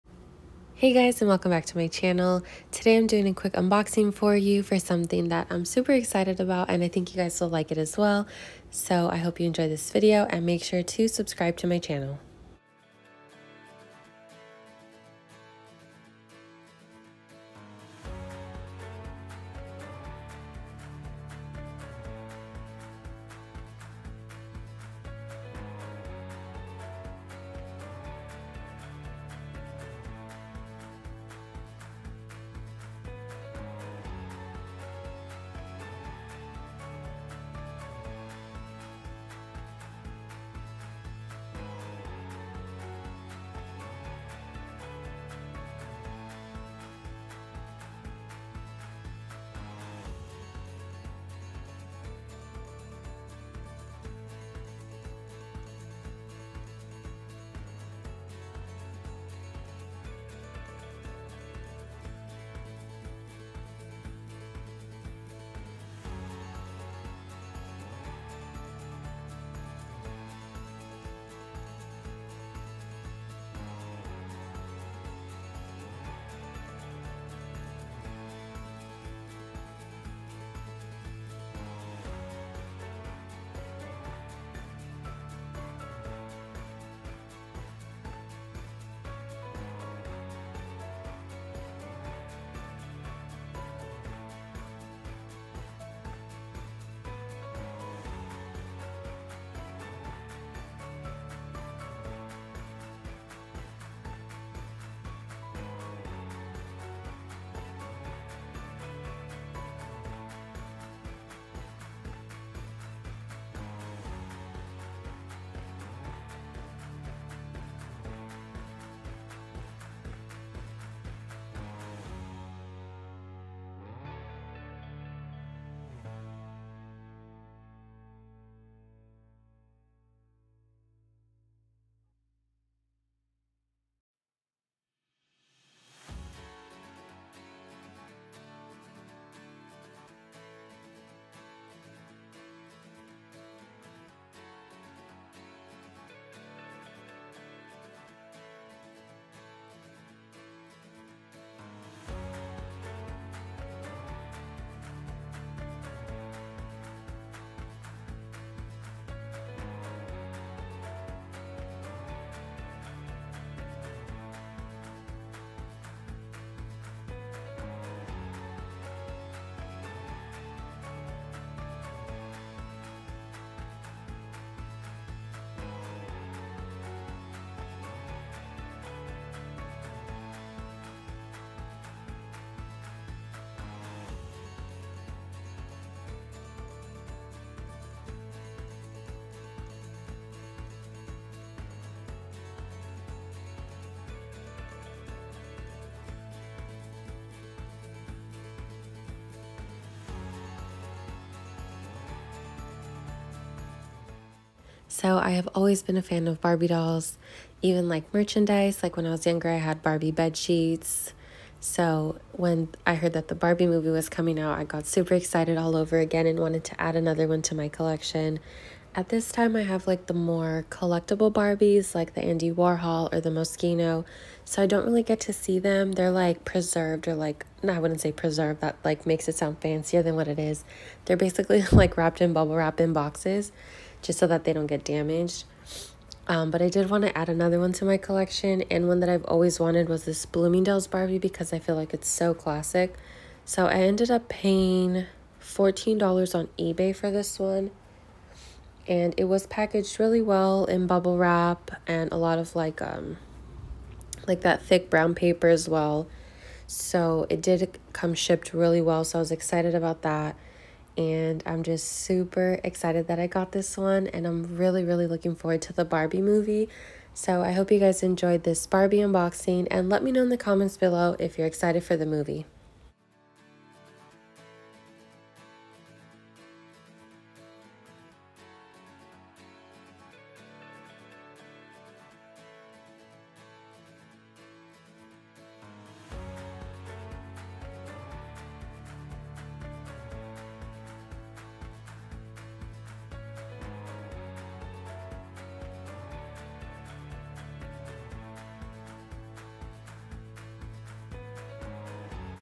I Barbie Series I Unboxing the cutest and stylish Barbie doll, just in time for the movie!
Comment and let me know if you’re excited for the Barbie movie like me?!

Happy Tuesday!

Britt